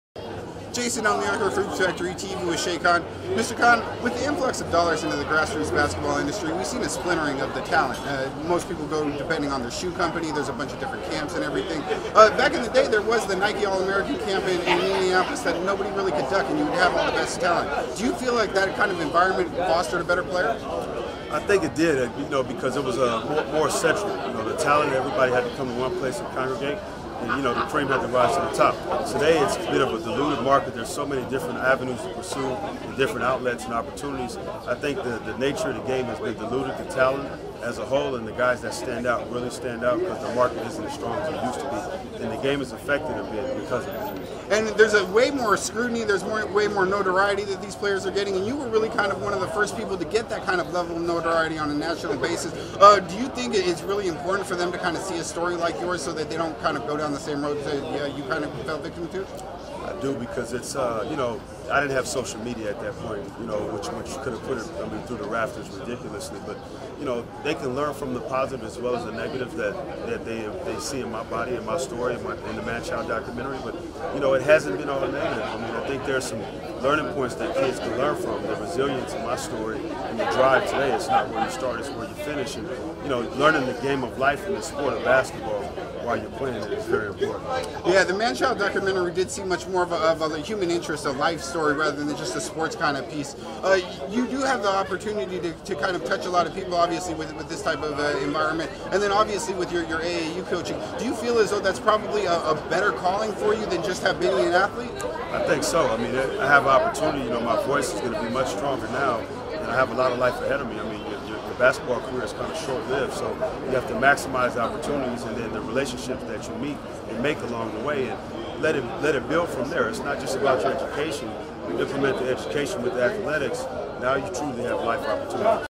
Schea Cotton Interview #2 @ Manchild Screening
Hoops Factory TV was lucky enough to attend the 2nd Los Angeles Screening of Manchild: The Schea Cotton Story.  After the screening, we were privileged enough to catch up with the man himself, Schea Cotton.  The basketball prodigy opened up on the film, his journey and his current place in life.